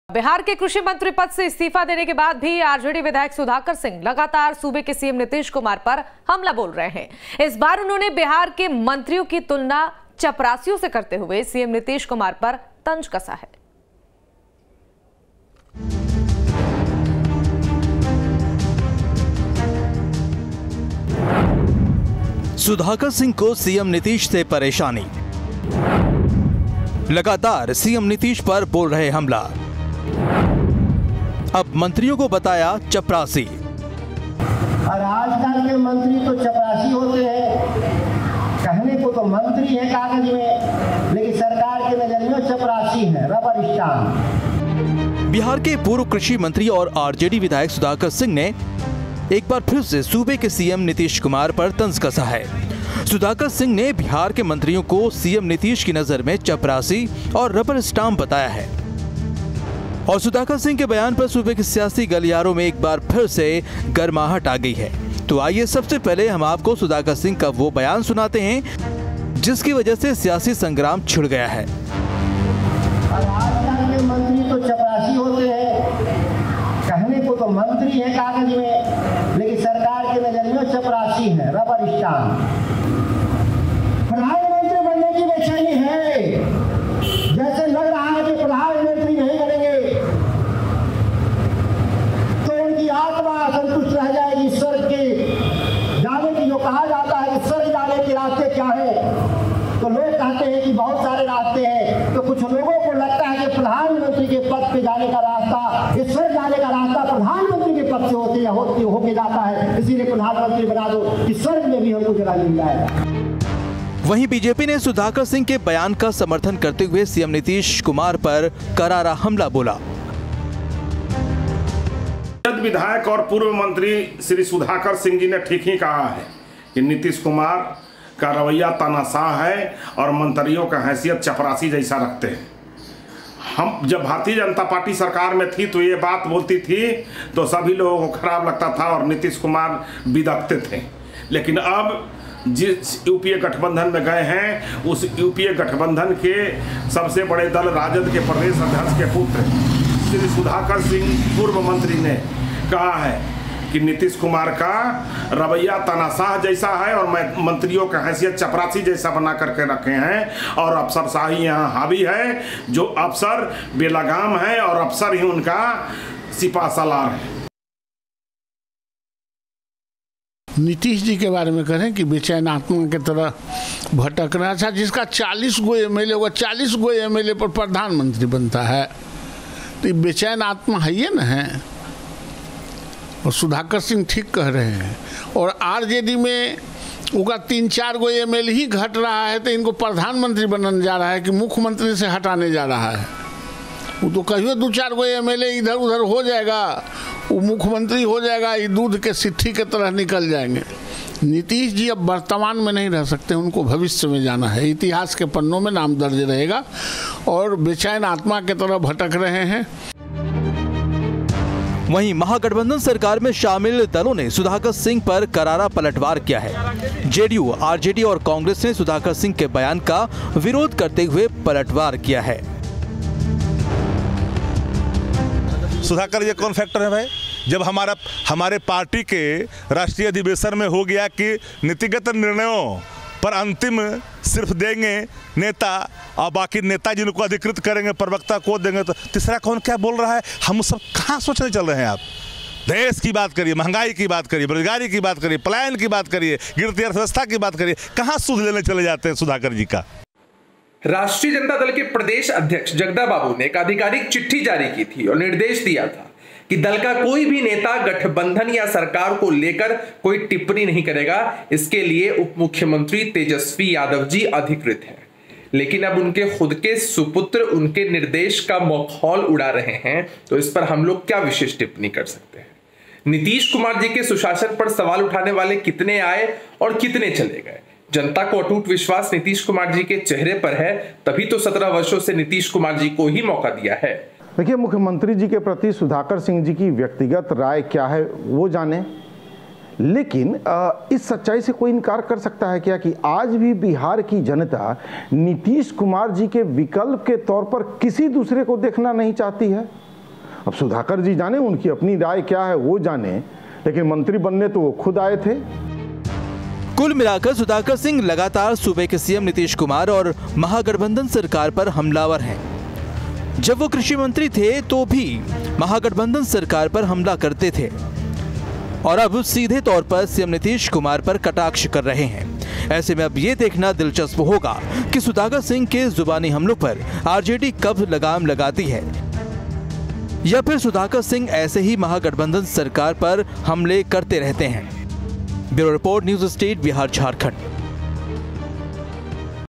सीएम नीतीश पर बरसे पूर्व कृषि मंत्री सुधाकर सिंह | Sudhakar Singh | CM Nitish Kumar | Bihar Politics
पूर्व मंत्री सुधाकर सिंह ने फिर से सीएम नीतीश कुमार पर हमला बोला है कि सुधाकर सिंह ने कहा है कि सीएम के नजर में सभी मंत्री चपरासी हैं...सभी कामों की देखरेख मुख्यमंत्री करते हैं बाकी लोग सिर्फ नाम के मंत्री बने हैं...सुधाकर सिंह यहीं नहीं रुके सुधाकर सिंह ने सीएम नीतीश के पीएम बनने की चाहत पर भी कहा कि 'कुछ लोगों में पीएम बनने की बेचैनी है' देखिए पूरी खबर-